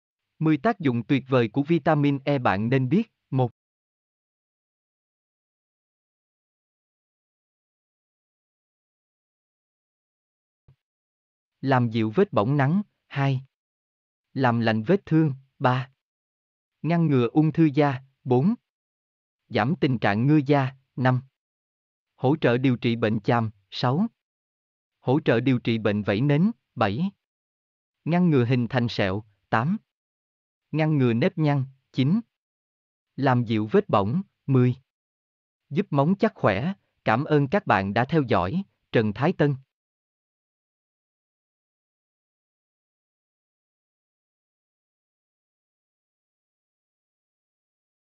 CÔNG DỤNG TUYỆT VỜI CỦA VITAMIN E I Siberian Wellness
10 tác dụng tuyệt vời của vitamin E bạn nên biết!

Vitamin E là một trong những loại vitamin tan trong dầu rất quen thuộc mà các bạn thường nghe nhắc tới. Loại vitamin này nổi tiếng với công dụng chống oxy hóa và kháng viêm. Vì vậy nó đem lại rất nhiều lợi ích cho cơ thể nói chung và làn da nói riêng. Trong bài viết này, hãy cùng tìm hiểu tác dụng của vitamin E và cách sử dụng chúng hiệu quả và an toàn qua bài viết
Làm lành vết thương
Nhiều nghiên cứu cho thấy rằng viên uống vitamin E thúc đẩy nhanh quá trình làm lành những tổn thương. Vitamin E dạng dầu cũng có thể đem lại công dụng tương tự, tuy nhiên vẫn còn ít nghiên cứu đối với dạng này.

Ngăn ngừa ung thư da
Như chúng ta đã biết tia cực tím trong ánh sáng mặt trời là yếu tố có thể làm khởi phát bệnh ung thư da. Một nghiên cứu được thực hiện năm 2013 cho thấy rằng bổ sung vitamin E giúp ngăn ngừa ung thư da. Tác dụng của vitamin E là giúp chữa lành những thương tổn ở tế bào khi tiếp xúc với ánh sáng mặt trời. Nhờ vậy, tế bào da được chữa lành và không biến đổi thành các tế bào ung thư.

Giảm tình trạng ngứa da
Sự thật là vitamin E không có khả năng chữa trị tình trạng dị ứng, nhiễm trùng hay các nguyên nhân khác gây ngứa da. Tuy nhiên, nhờ vào công dụng dưỡng ẩm của nó mà giúp giảm nhanh triệu chứng ngứa ngáy khó chịu. Các bạn cần lưu ý rằng, khi da khô thì tình trạng ngứa sẽ trở nên trầm trọng hơn. Vì thế dưỡng ẩm đầy đủ cho da biện pháp giúp hỗ trợ giảm ngứa.

Hỗ trợ điều trị bệnh chàm
Khi mắc phải bệnh lý chàm hay viêm da cơ địa. Người bệnh thường gặp phải tình trạng da khô, sần sùi hoặc bong tróc thường xuyên. Một nghiên cứu được thực hiện cho thấy rằng viên uống vitamin E có thể giúp làm giảm các triệu chứng của chàm.

Hỗ trợ điều trị bệnh vảy nến
Một nghiên cứu khác cho thấy dầu vitamin E giúp làm giảm các triệu chứng của bệnh lý vảy nến. Đáng chú ý hơn là cũng nghiên cứu đó cho thấy nó không gây bất kỳ tác dụng phụ nghiêm trọng nào khi sử dụng trên người bệnh vảy nến. Tuy nhiên, tác dụng của vitamin E không tốt bằng các loại sản phẩm điều trị đặc hiệu khác dành cho bệnh nhân vảy nến. Dạng dầu vitamin E chỉ có thể nên sử dụng cho những người mắc bệnh nhẹ, không muốn sử dụng như biện pháp đặc hiệu thường xuyên.
Ngăn ngừa hình thành sẹo
Các chuyên gia cho rằng sử dụng viên uống hay dầu vitamin E có công dụng ngăn ngừa hình thành sẹo. Tuy nhiên, nhiều nghiên cứu lại cho thấy rằng vitamin E không giúp ngăn ngừa sẹo. Một nghiên cứu vào năm 1999 còn cho thấy bị dị ứng với vitamin E còn làm cho tình trạng sẹo trầm trọng hơn. Từ đó, nhiều nghiên cứu được thực hiện nhưng vẫn chưa đưa ra kết luận vitamin E có giúp ngăn ngừa sẹo hay không.

Tuy nhiên, nhờ vào những chứng minh rằng dưỡng ẩm tốt cho da giúp giảm hình thành sẹo nên đối với những ai không dị ứng với vitamin E thì sử dụng nó giúp dưỡng ẩm, nhanh lành vết thương và ít tạo sẹo hơn.

Ngăn ngừa nếp nhăn
Da khô dễ xuất hiện nếp nhăn hơn so với da đủ ẩm. Vì vậy, nhờ vào công dụng dưỡng ẩm tuyệt với của vitamin E mà giúp da trông căng đầy và ít nếp nhăn.

Làm dịu vết bỏng nắng
Nhờ vào tác dụng dưỡng ẩm mà vitamin E giúp làm dịu tình trạng da bị khô, bỏng rát và kích ứng khi bị bỏng nắng. Tác dụng của vitamin E còn giúp bảo vệ khỏi các gốc oxy hóa gây hại. Tuy nhiên, chỉ sử dụng vitamin E không giúp bảo vệ da hoàn toàn khỏi ánh nắng mặt trời. Các bạn cần trang bị thêm vật dụng và kem chống nắng để bảo vệ da một cách hoàn hảo nhất.

Giúp móng chắc khỏe
Bổ sung viên uống vitamin E giúp giảm tình trạng móng vàng, móng dễ gãy. Dầu vitamin E cũng giúp dưỡng ẩm cho móng và giúp móng thêm chắc khỏe.